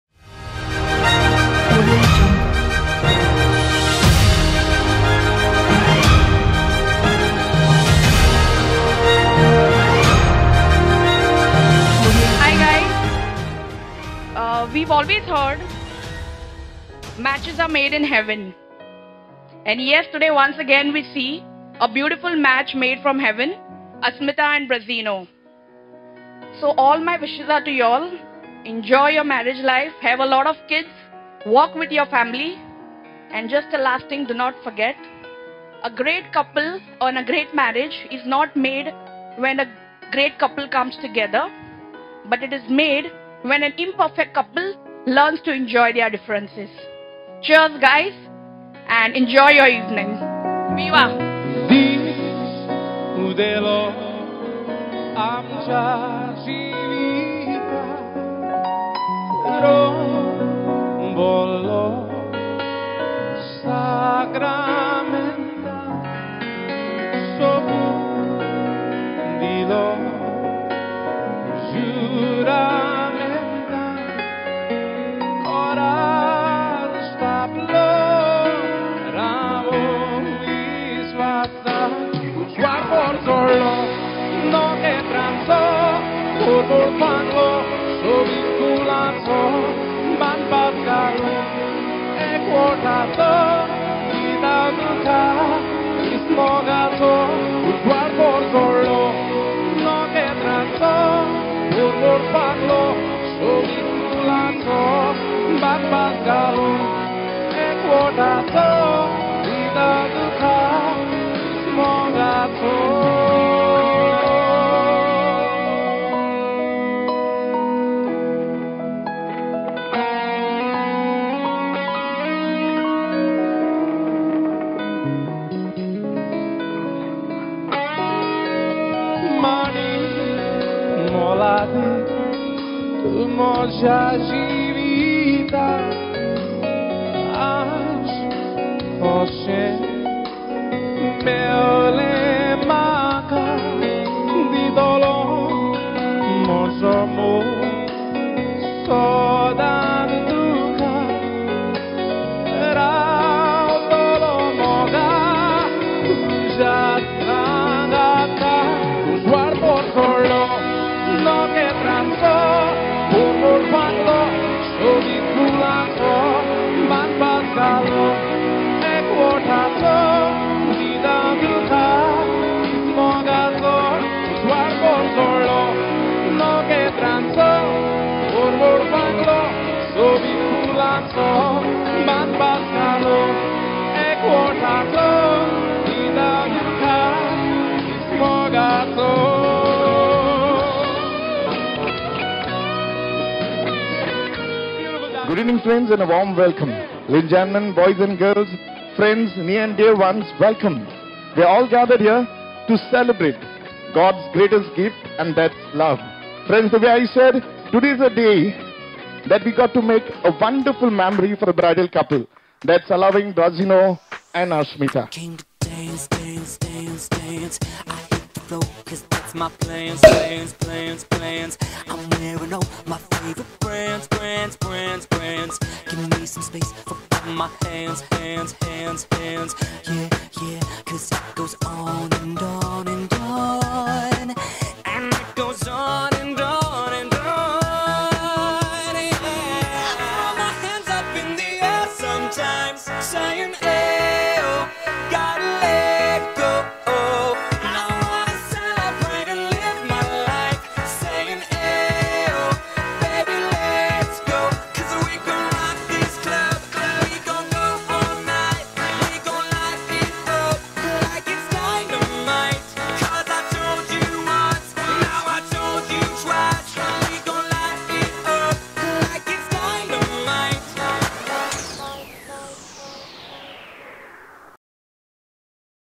Goan wedding of Brazinho & Asmita on 19th Nov. 2022
Reception at Menezes lawns Bastora - Goa.
Video and Photo by Angel Production